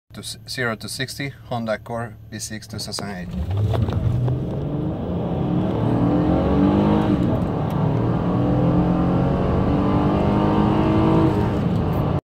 2008 Honda Accord V6 0 to 60
2008 Honda Accord EXL V6 0 to 60 in 6.25 seconds